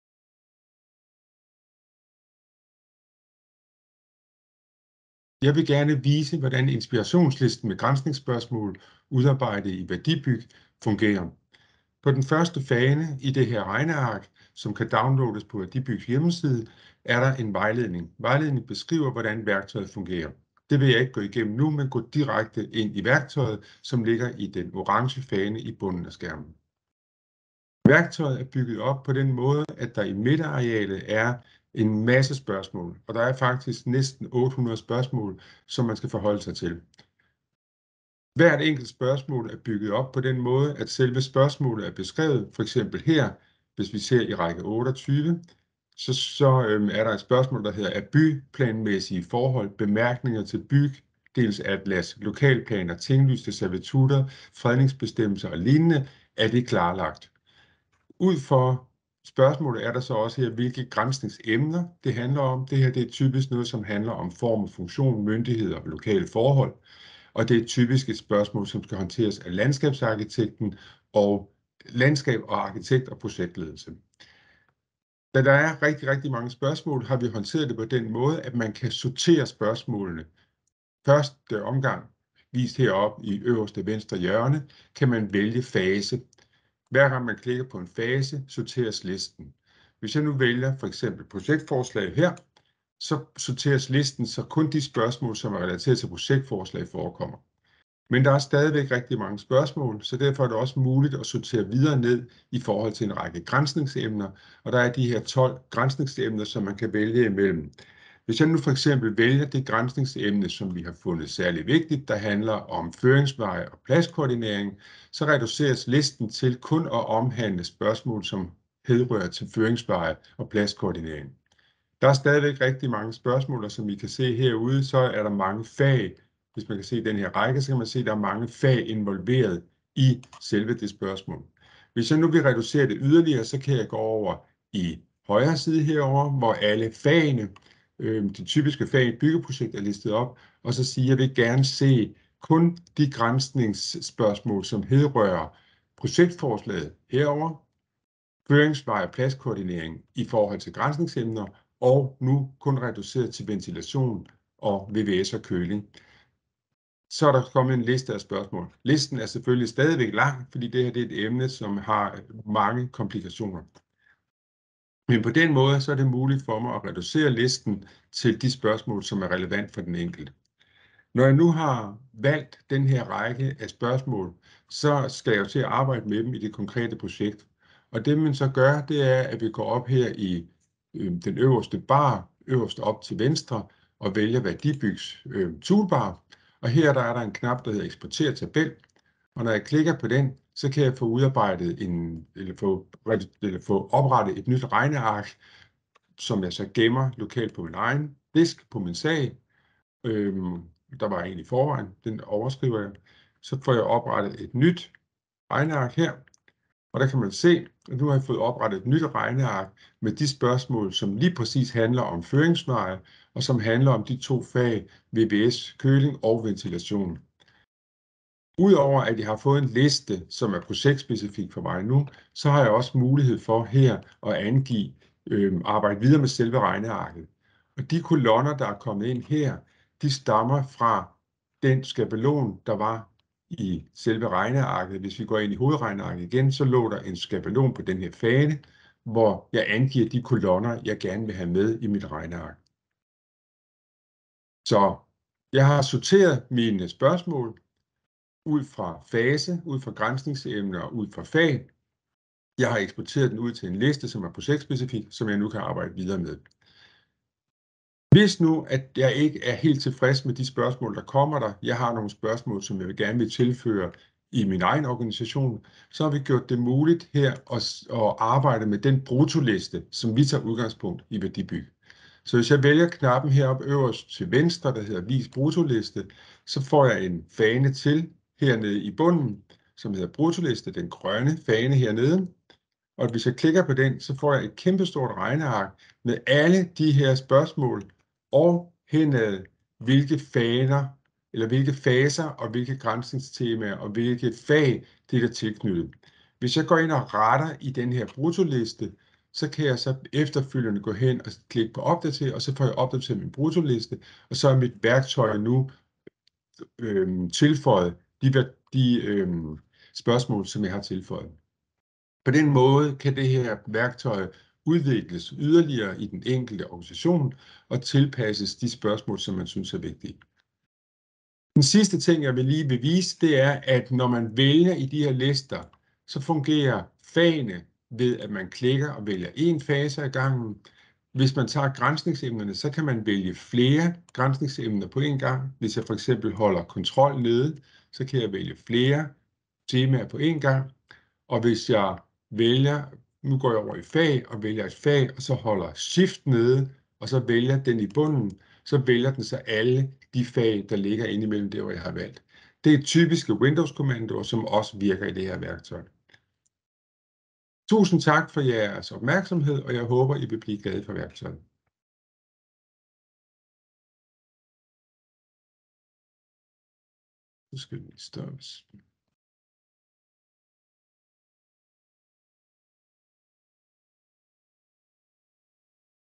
Værktøj til Projektgranskning
Her kan du få en lille guide til Værdibygs værktøj til projektgranskning. Lær, hvordan du nemt og hurtigt finder rundt og får den optimale værdi ud af værktøjet.